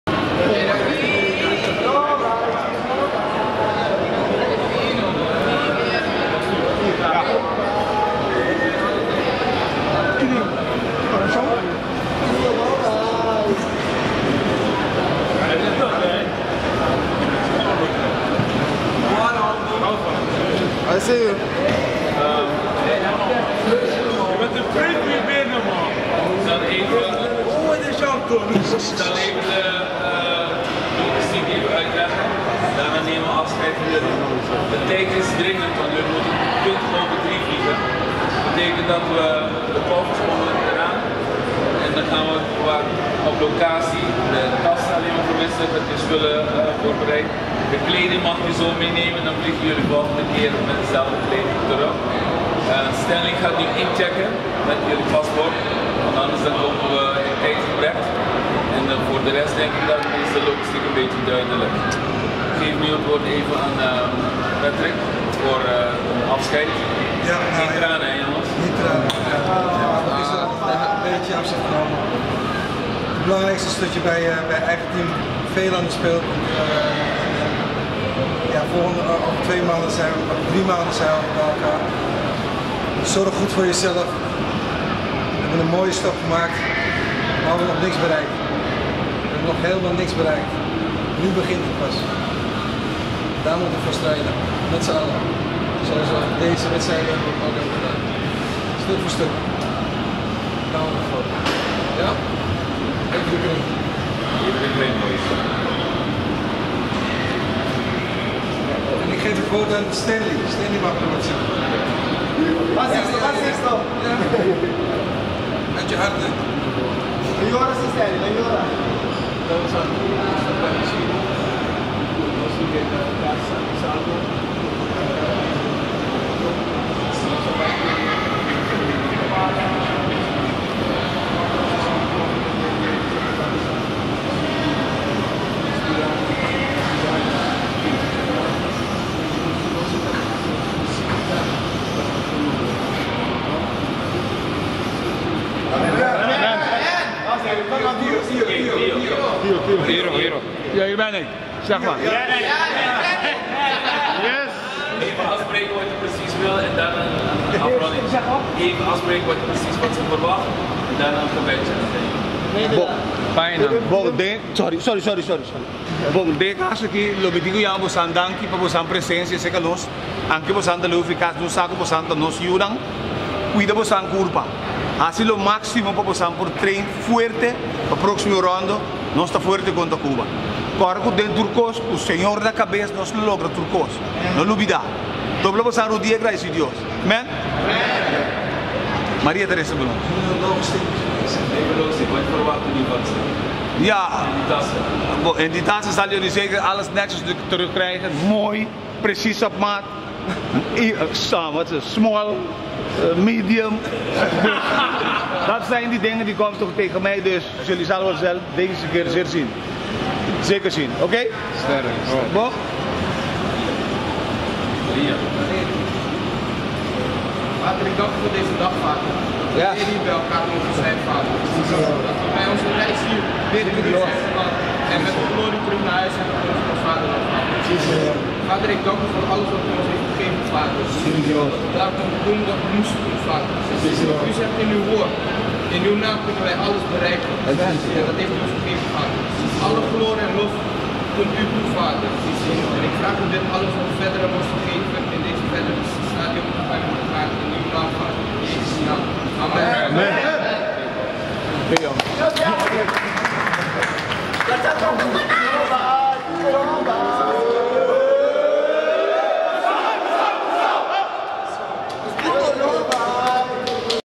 Curacao top klousura
Curacao ta klousura e viahe pa Montserrat yamando Dios Tata danki den orashon.

One Love.